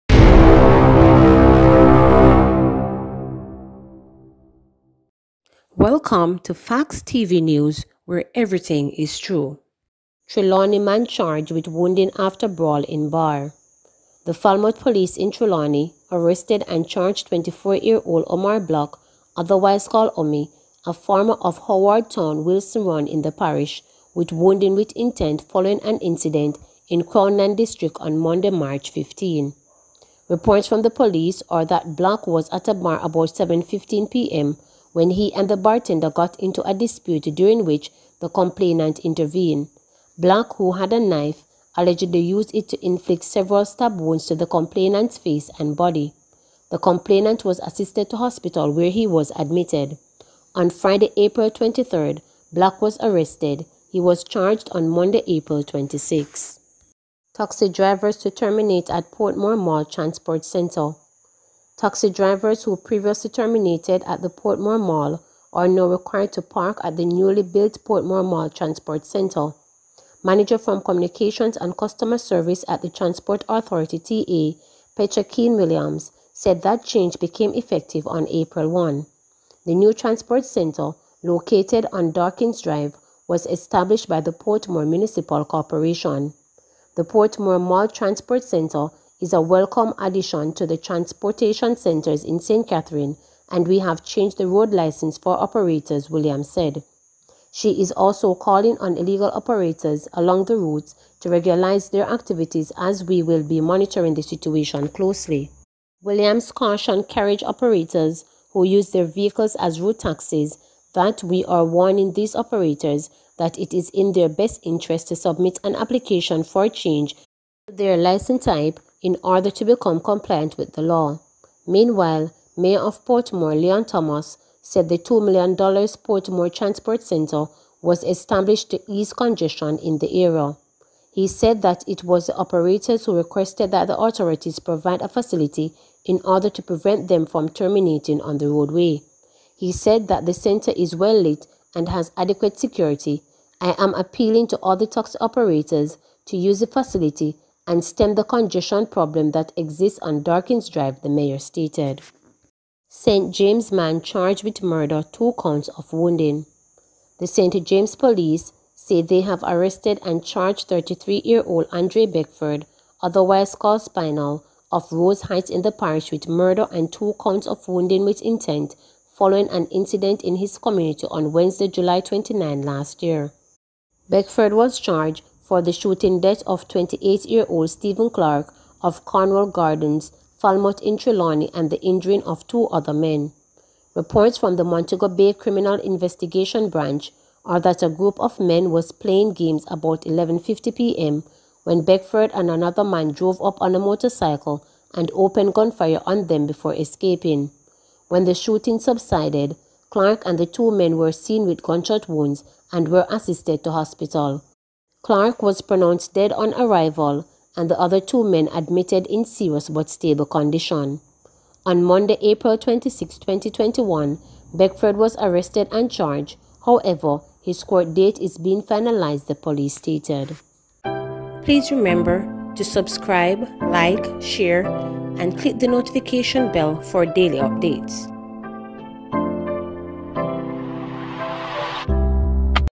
Facts TV News April 27, 2021
Man on st@bbing charge/ Taxi driver urges to use portmore mall transport center